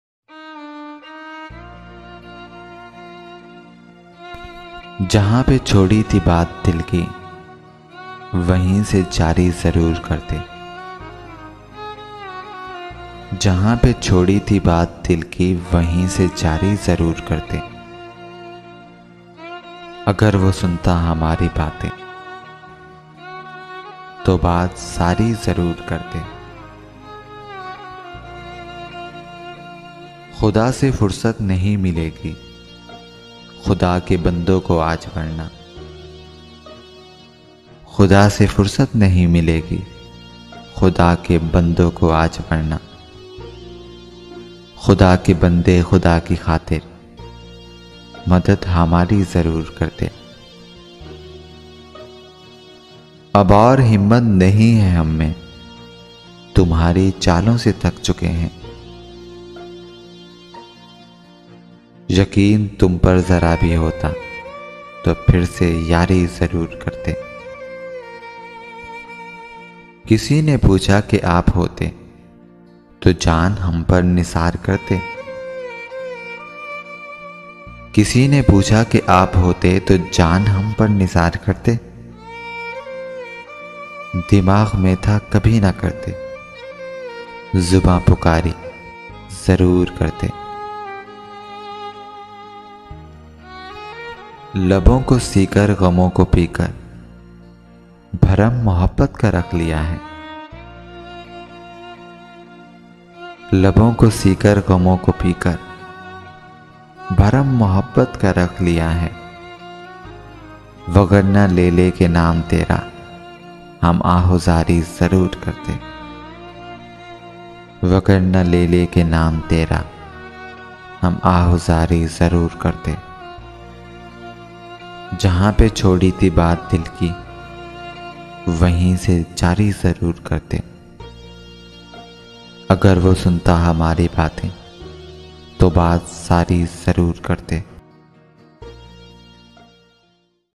Best Urdu poetry l jahan pay chori thi baat Dil ki l sad urdu poetry l Ramz e Urdu
Jahan pay chori thi baat dil ki wahin say jari zaroor kartay

Agr wo sunta  humari batain to baat sari zaroor kartay

Narrator-Qadeer Hussain

Poet-M.Raza Hyderi

Label-Ramz e Urdu